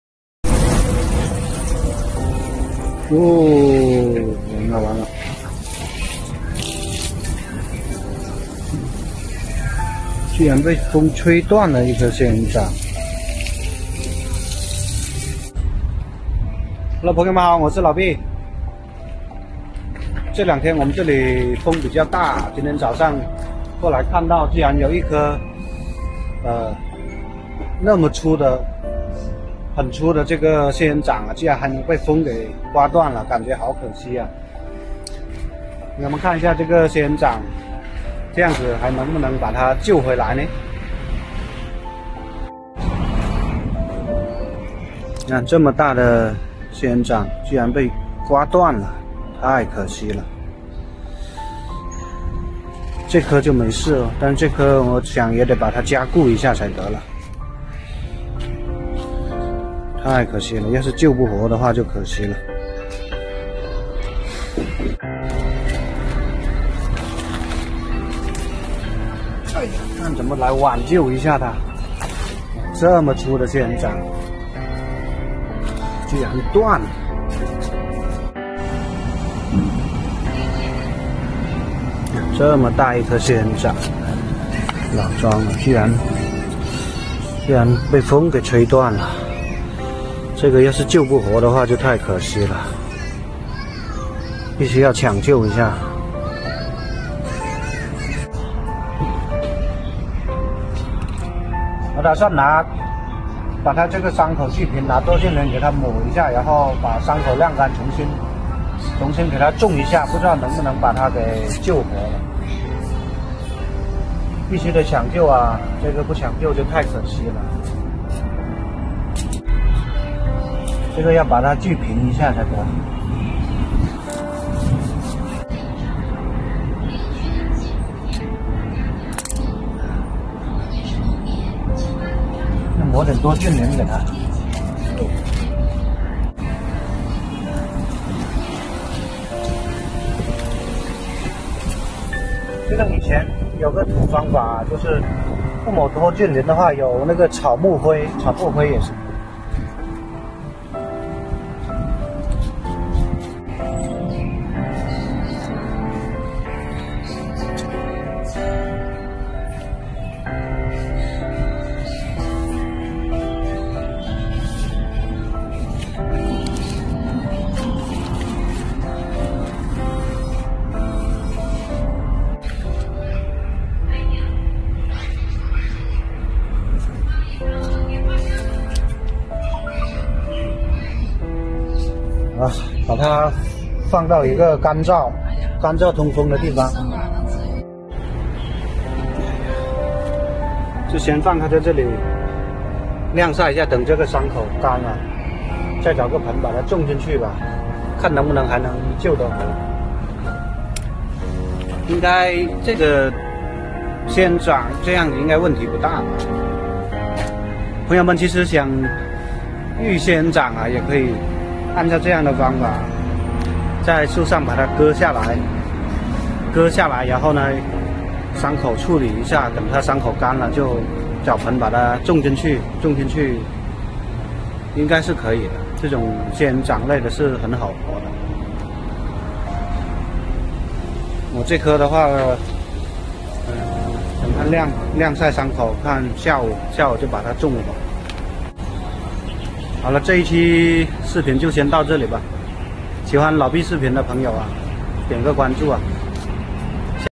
大仙人掌被大风吹断，老闭第一时间给它抢救，这样还能救回来吗
大仙人掌被大风吹断，老闭第一时间给它抢救，这样还能救回来吗,本视频由阿祖养花vlog原创提供,70次播放,好看视频是由百度团队打造的集内涵和颜值于一身的专业短视频聚合平台